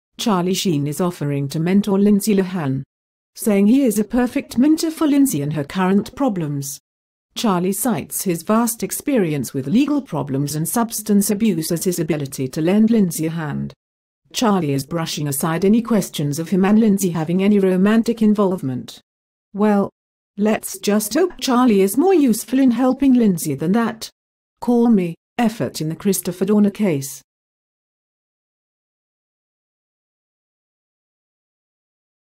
Charlie Sheen and Lindsay Lohan
Charlie offers to mintor Lindsay.